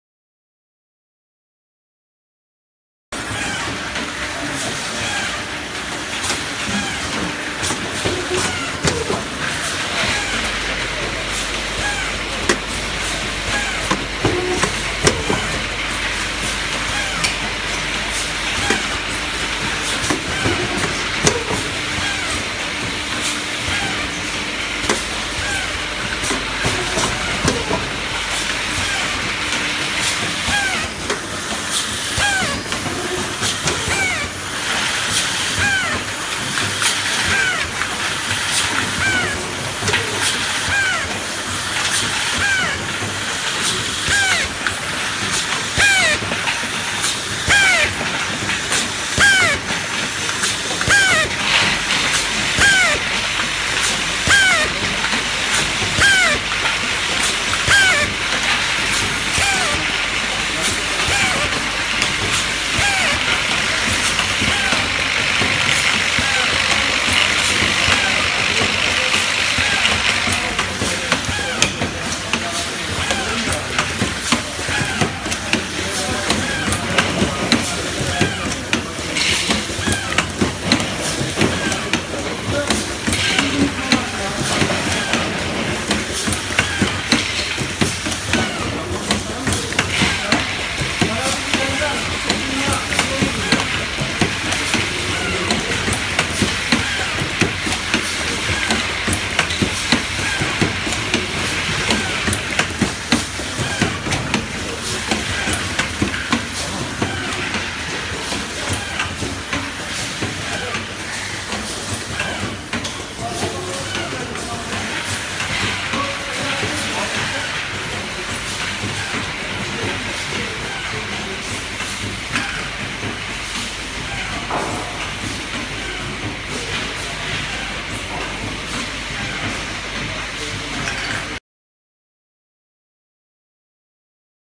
Ayakkabı Boyası Hattı - Shoe Polish Line
Bereketsan Makina; dolum makineleri, kapak kapatma, etiketleme ve ambalajlama üzerine makineler üretmektedir. Bu video Ayakkabı Boyası Hattı - Shoe Polish Line çalışma prensibi göstermek için Bereketsan Makina tarafından yayınlanmıştır. Makine ile ilgili diğer tüm detaylara internet sitemizden ulaşabilirsiniz